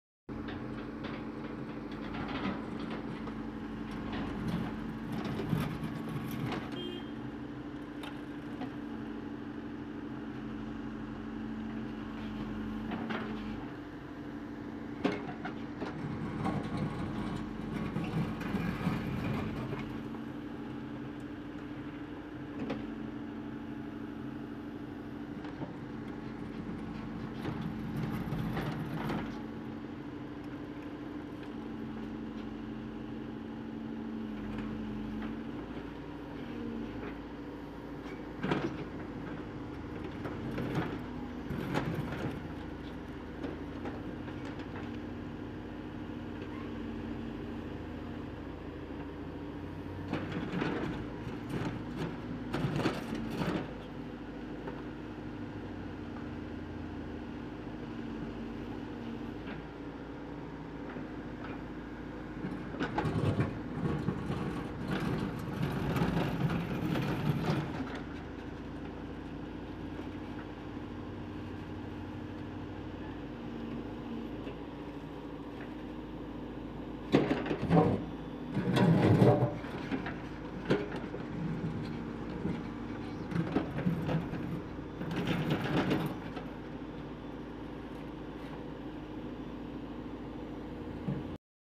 jcb work in dubai warqa site
is video ma aap jcb ksy chalaty hain.seekh sakain gy.